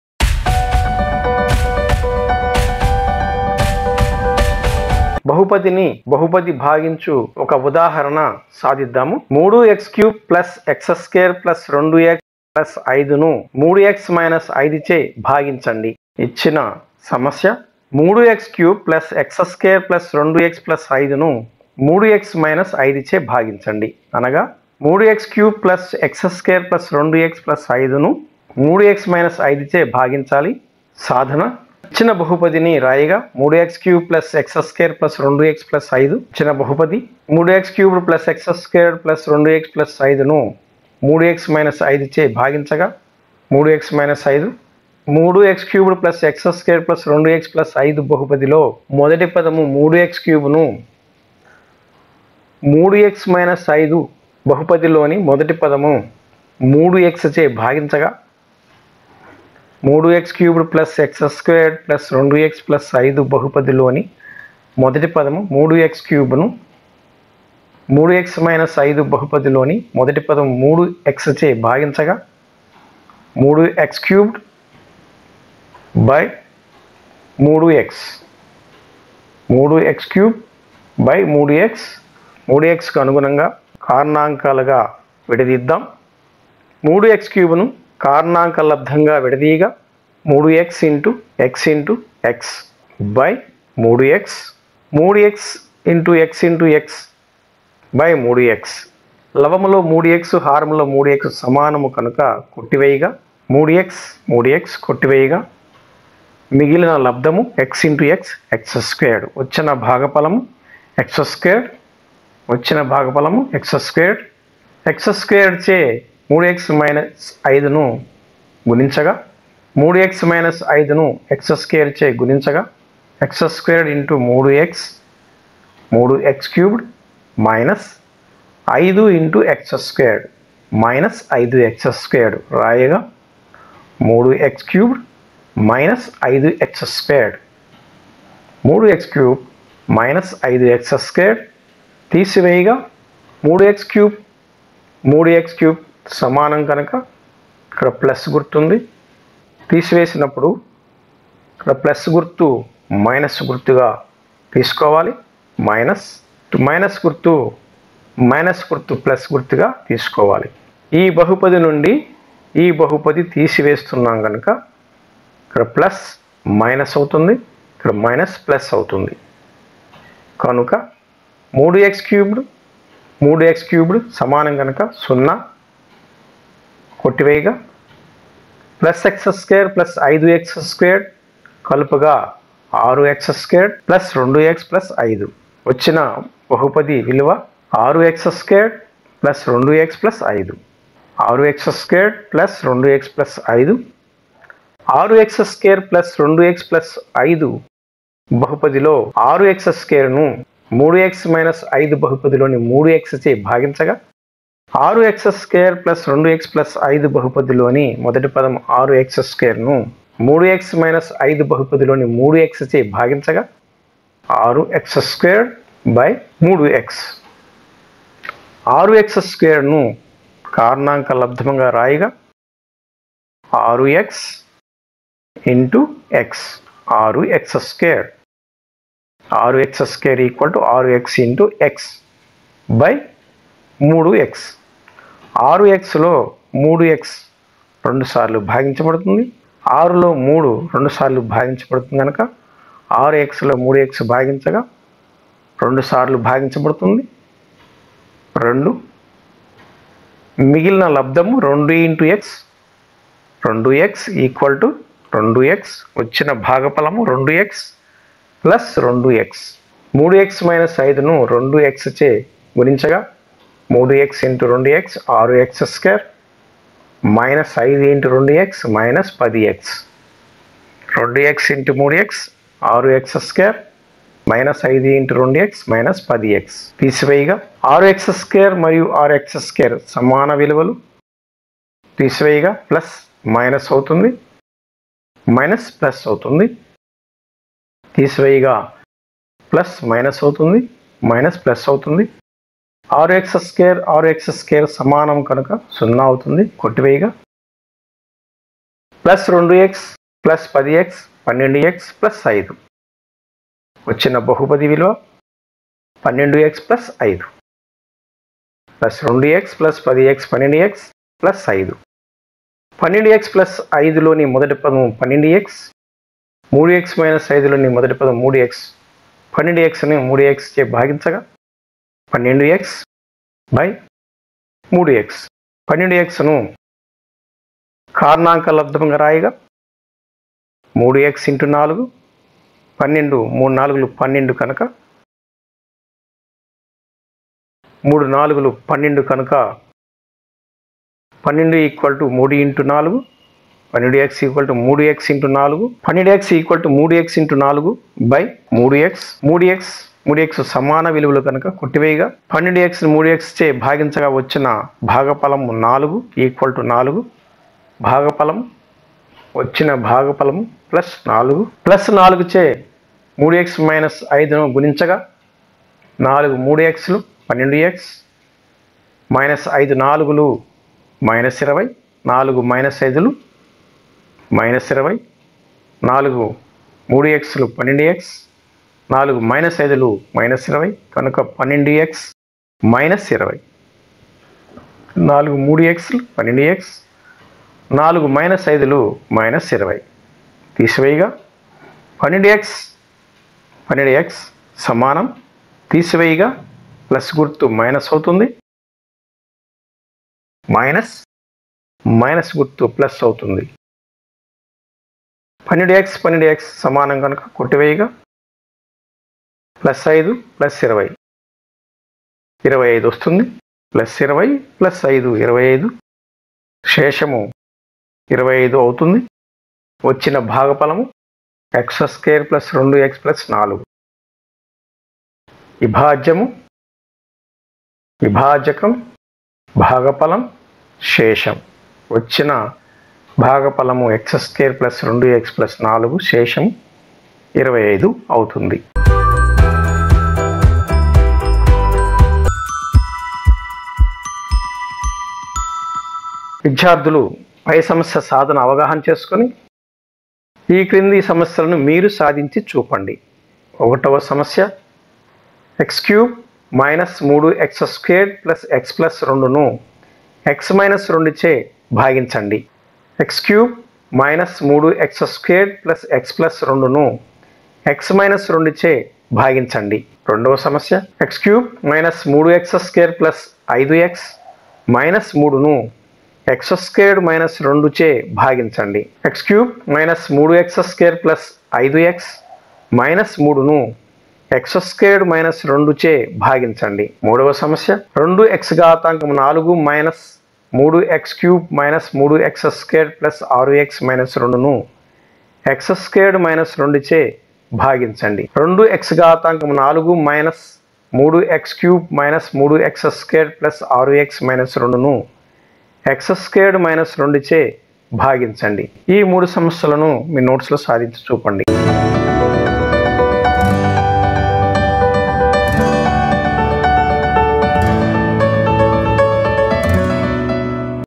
Polynomials-Division/solutions బహుపదులు-భాగహారము/ బహుపదిని బహుపది భాగించు సమస్యలు సాధన
బహుపదులు- భాగహారము/ బహుపదిని బహుపది భాగించు సమస్యలు సాధన-8 వ, 9 వ మరియు 10 వ తరగతుల విద్యార్థులకు తెలుగు మీడియము విడియో పాఠాలు nsrmaths@practice